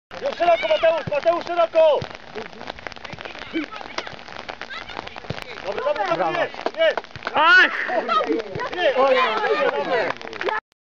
2008.10.04 Sęp Żelechów (trampkarze) - Tęcza Stanisławów: Sęp w spojenie
Niewiele brakowała a kilka minut po zdobyciu drugiej bramki mogła paść kolejna. Po strzale z dystansu piłka uderzyła jednak w spojenie słupka i poprzeczki.